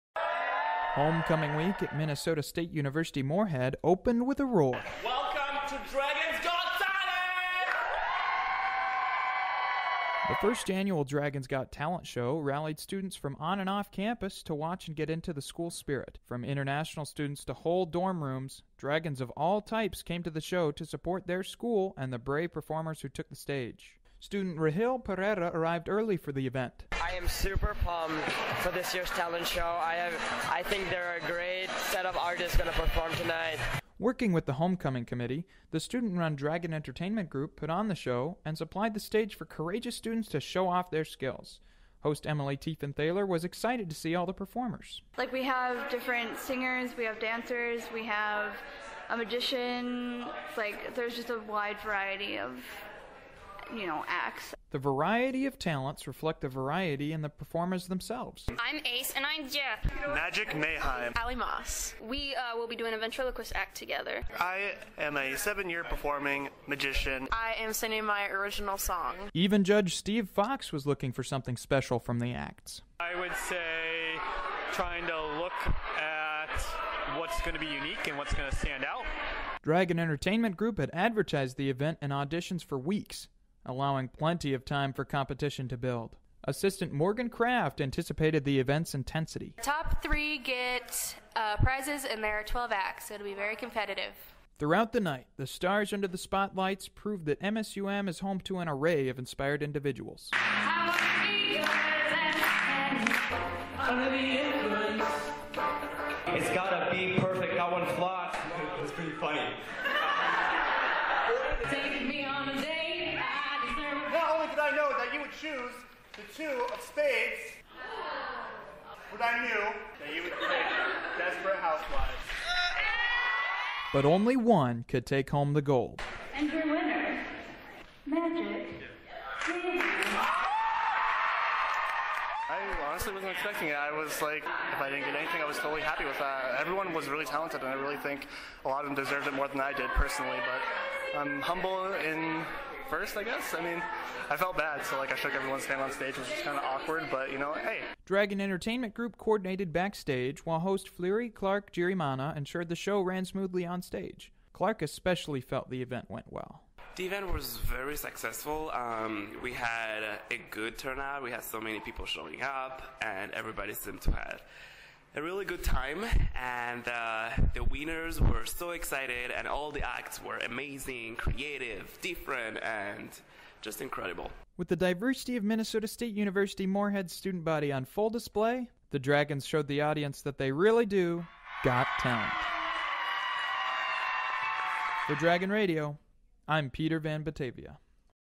Dragons Got Talent
Students at MSUM share their talent at the univeristy's annual competition. Aired on 12-5-18 on Minnesota State University Moorhead's Dragon Radio.